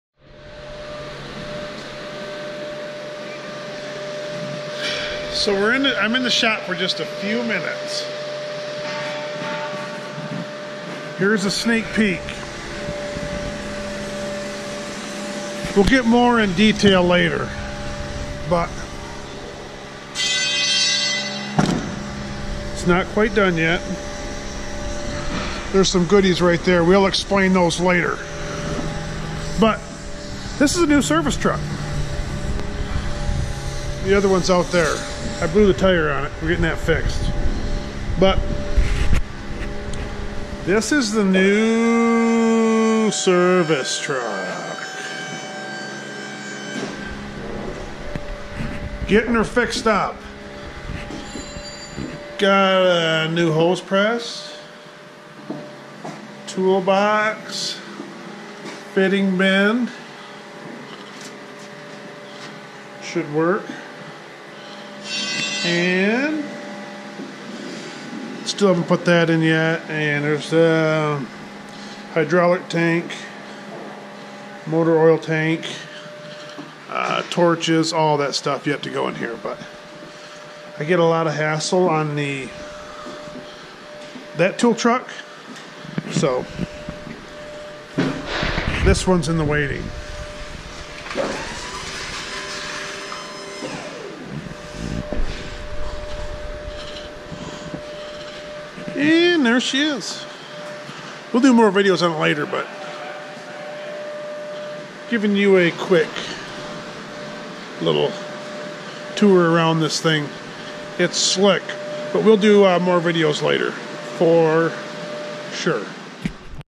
NEW TOOL TRUCK TEASER!!
Thought I would give ya a little new tool truck teaser! We will be doing some more videos on this new service truck. This thing is sharp!! New Frieghtliner in the shop!! This thing is still getting set up but once it's all ready it should work out really nice. She is a brand spanking new Frieghtliner Box Truck. More to come...hope y'all enjoy!!